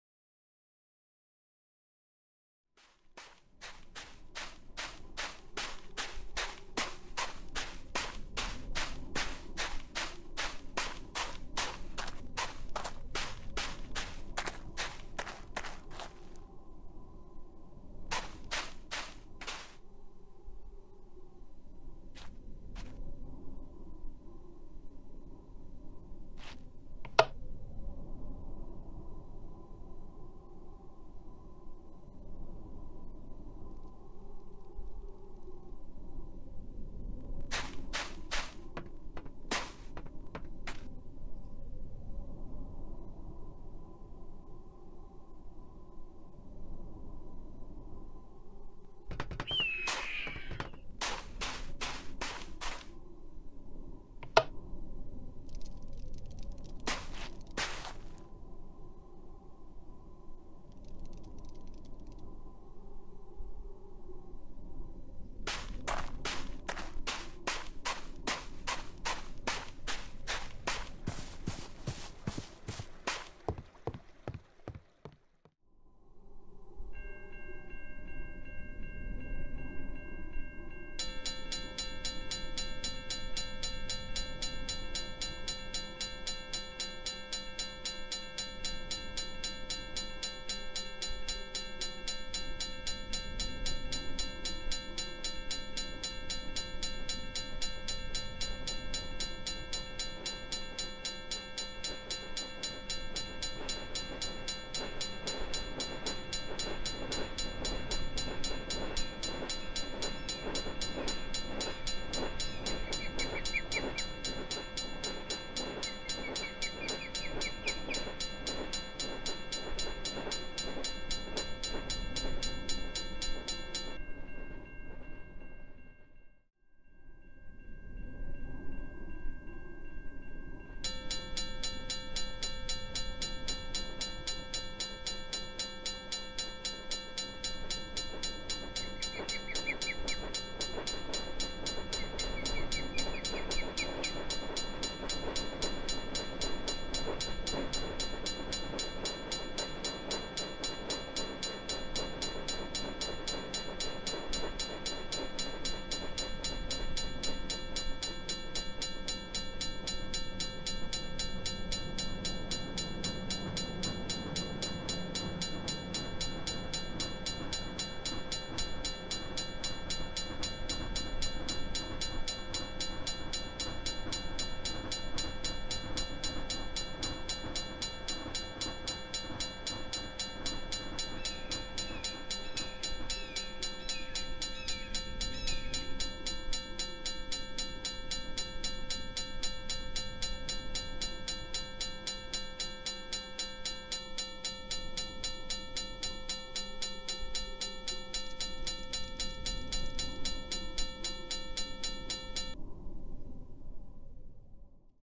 Bahnübergang manuelle Schranke
Das ist ein manuell bedienter Bahnübergang. Es funktioniert wie eine Anrufsschranke, nur dass man anstelle eines Fernsprechers (der mit dem Stellwerk verbunden ist) einen Hebel hat, der die Schranken öffnet oder schließt. Da hier nur Bauern oder Spaziergänger unterwegs sind, reicht das aus. Die Lampen kommen noch von früher, wo die Anlage noch normal geschaltet war. Sobald sich ein Zug nähert, ertönt das Läutwerk und warnt so die Personen, die den BÜ überqueren möchten.

Züge:
-AMTRACK von Groß-Stadt Süd HBF nach South Hollows HBF
-Güterzug von South Hollows Güterbahnhof nach Reufeld
-Güterzug von Reufeld nach South Hollows Güterbahnhof